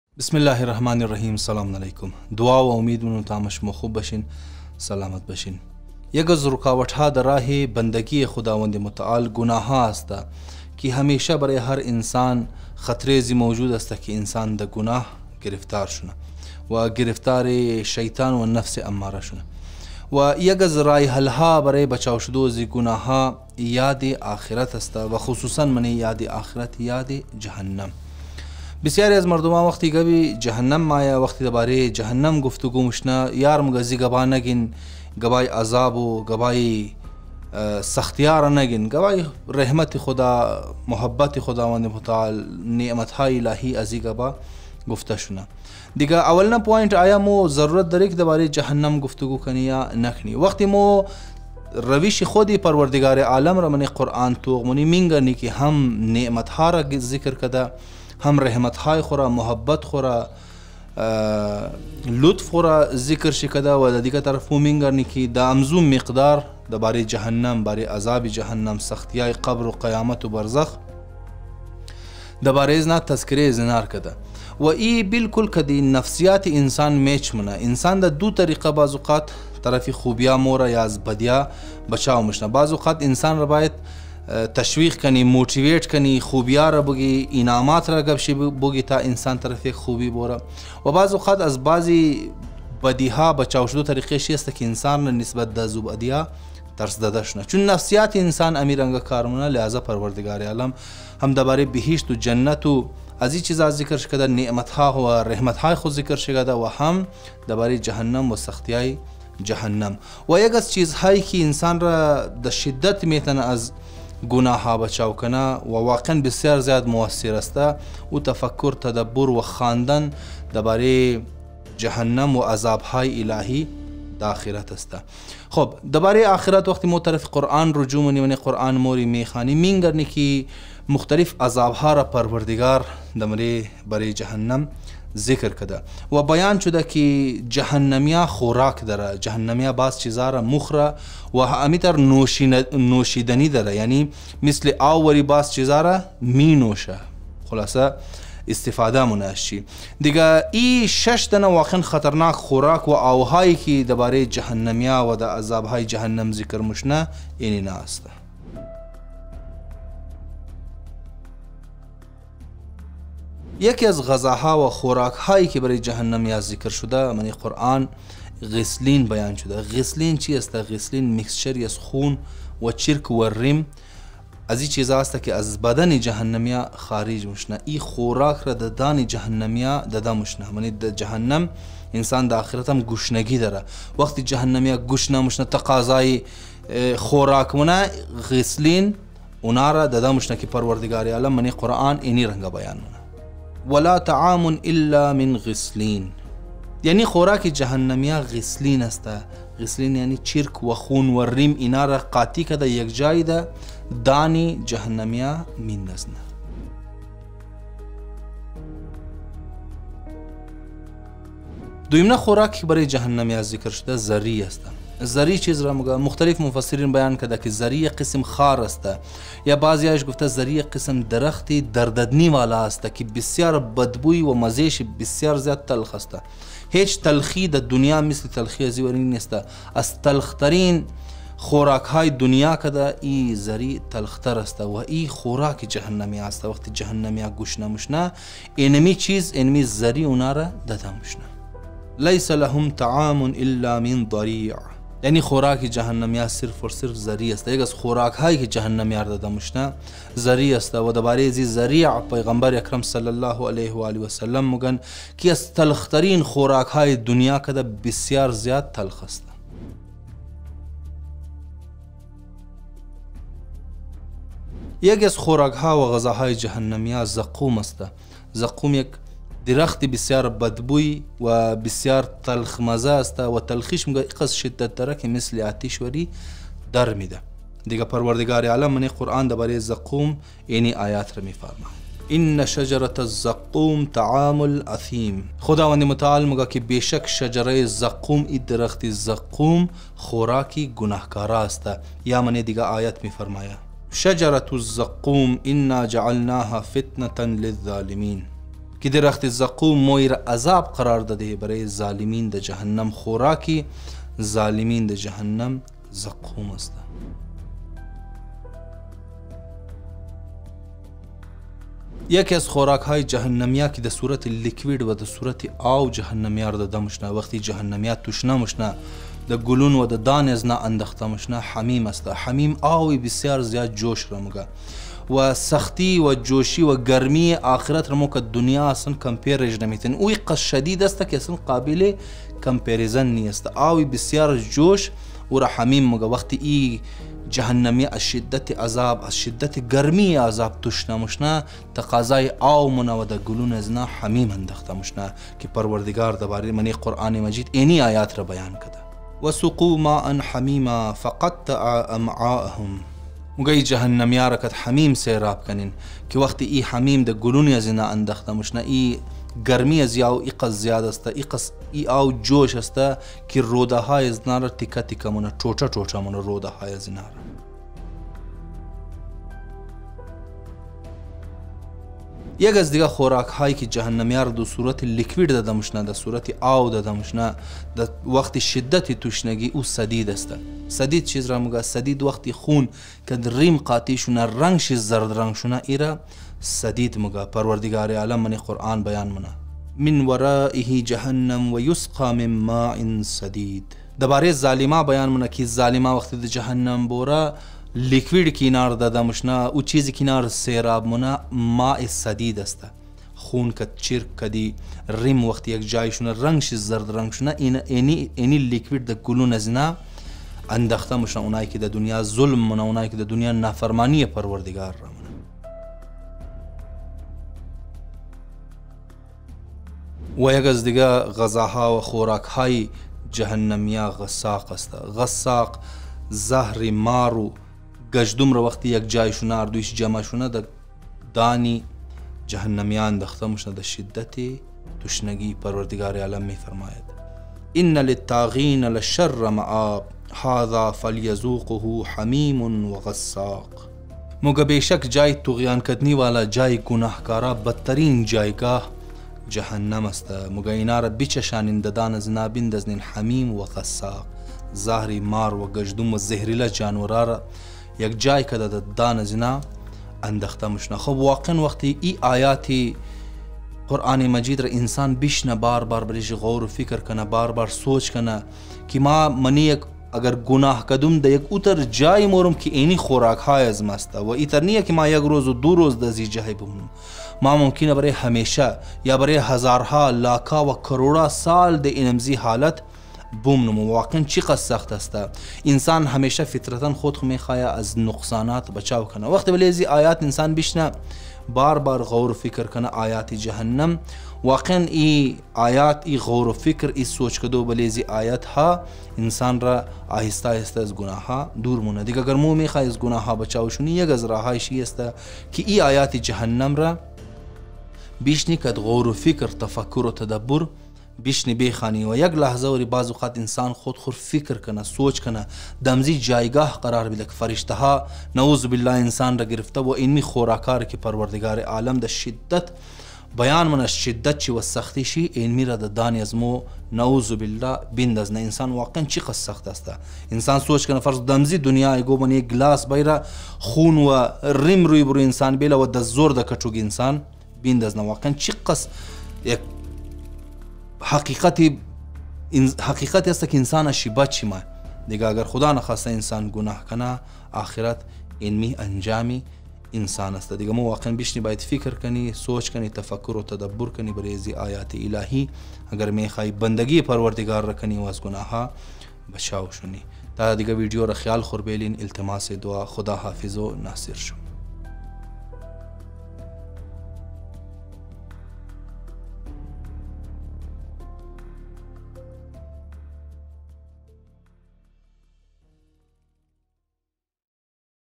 شش غلیظ ترین خوراک برای جہنمی ها کہ قرآن بیان کده
آخرت یک زندگی ہمیشگی استہ
و اگر انسان خدا نخواستہ اہل دوزخ تیار شونہ
سخت ترین عذاب ها و غلیظ ترین خوراک ہا نصیب شی موشنہ
شش غلیظ ترین خوراک برای جہنمی ہا کہ قرآن بیان کده